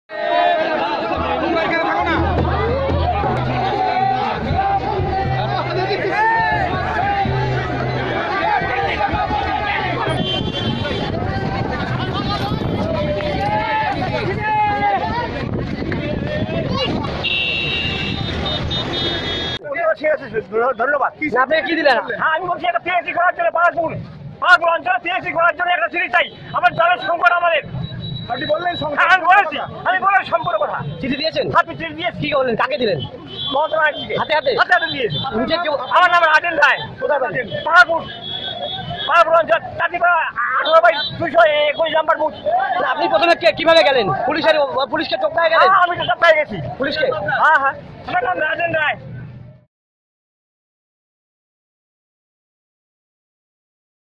Old farmer from Jalpiguri hands over a letter to Mamata Banerjee
মুখ্যমন্ত্রী যাবেন। আগে থেকেই তাই রাস্তার দু’পাশে সার বেঁধে অপেক্ষা করছিলেন প্রচুর মানুষ। ভিড় নিয়ন্ত্রণ করতে ছিল পর্যাপ্ত পুলিশকর্মীও। তার মধ্যেই পুলিশের নজর এড়িয়ে মমতার কাছে চলে গেলেন এক বৃদ্ধ। এলাকার পানীয় জলের সমস্যা সমাধানের জন্য সরাসরি মুখ্যমন্ত্রীর হাতেই চিঠি ধরিয়ে দিলেন জলপাইগুড়ির বাসিন্দা রাজেন রায়। এই ঘটনায় মুখ্যমন্ত্রীর নিরাপত্তা নিয়ে বড়সড় প্রশ্নও উঠে গেল। কিন্তু লাভের লাভ হল ওই এলাকার।